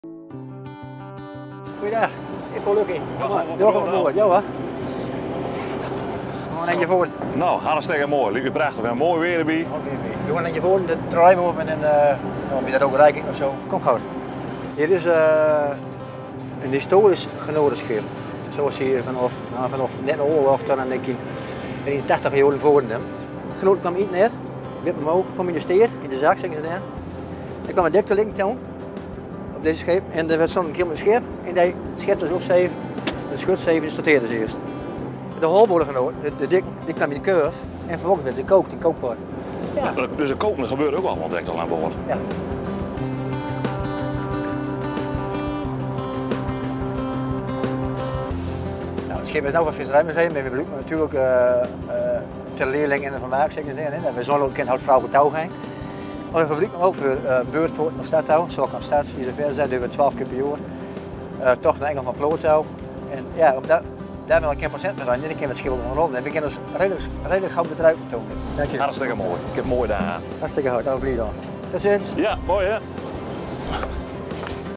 Varen op een historisch garnalenvissersschip in Zoutkamp
Het Groningse vissersdorp Zoutkamp was jarenlang afhankelijk van de garnalenvisserij. De historische garnalenkotter ZK 4 vaart tijdens de zomermaanden door het unieke Reitdiepgebied met de beurtvaart. Stoffer vaart een stukje mee richting het Lauwersmeer.